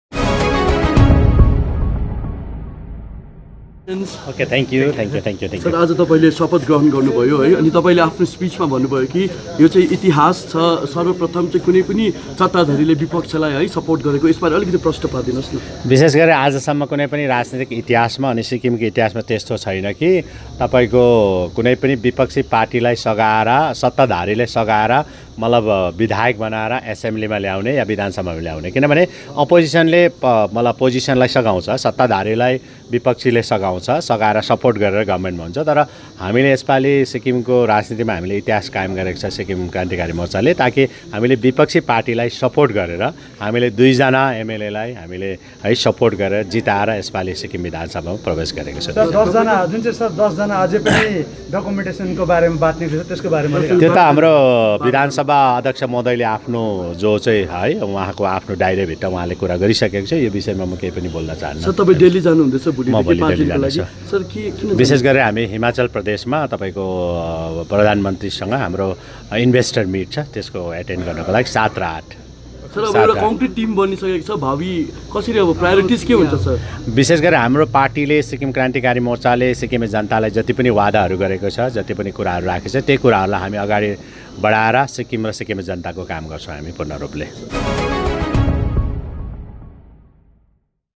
Chief Minister PS Tamang takes an oath as elected MLA
Chief Minister PS Tamang speaks with media after taking oath as the member of Sikkim Legislative Assembly